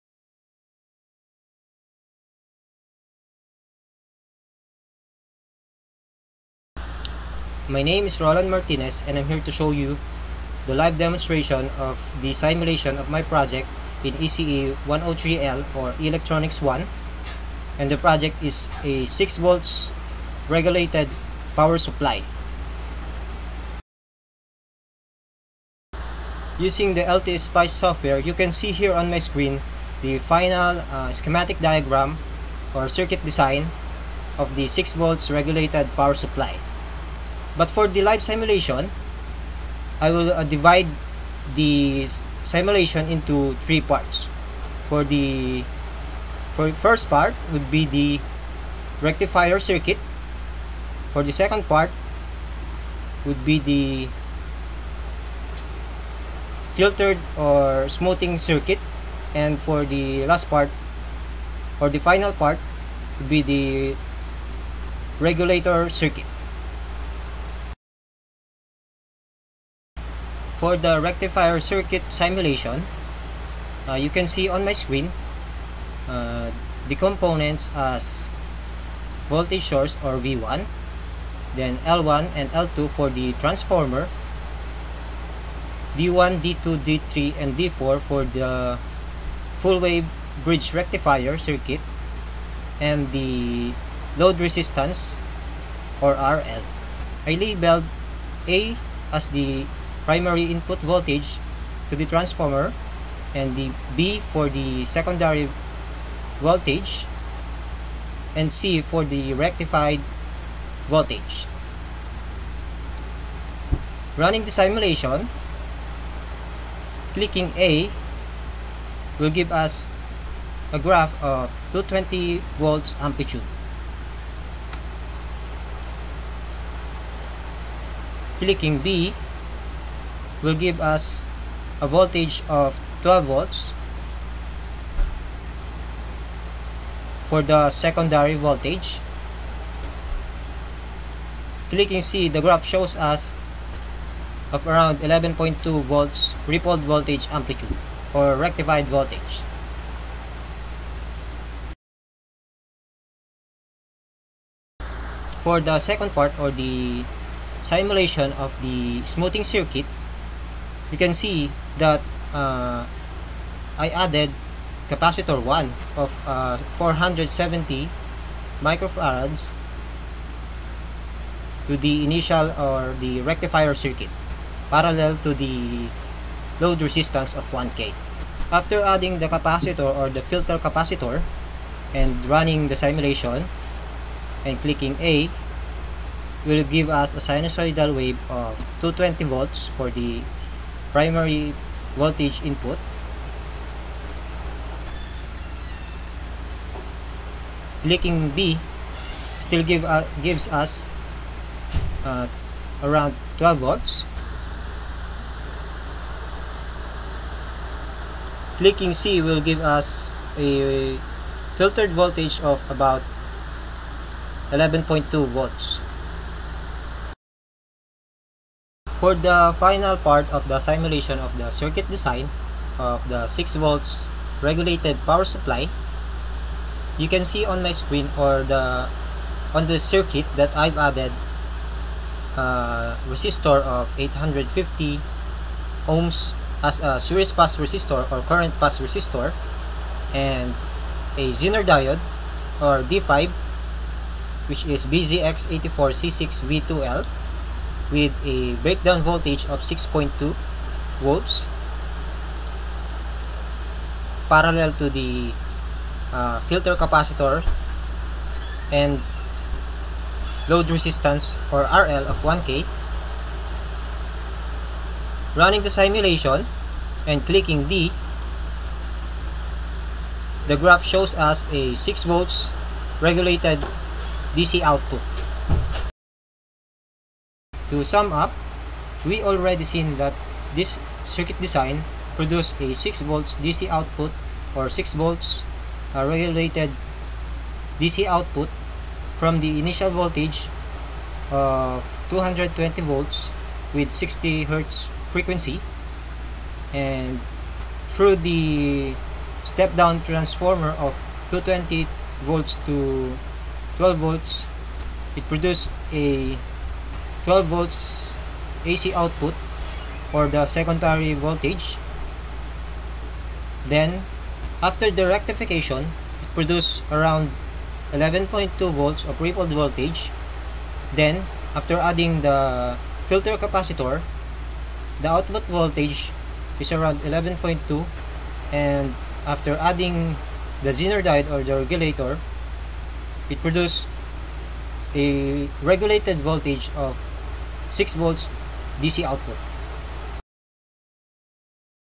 6VDC Regulated Power Supply Circuit Design Simulation
6VDC Regulated Power Supply Cicuit Design
Project in Electronics 1 Laboratory
00:00:00 Introduction
00:01:09  Rectifier Circuit Simulation
00:02:33  Rectifier Circuit Simulation
00:03:38  Regulator Circuit Simulation
00:04:35  Summary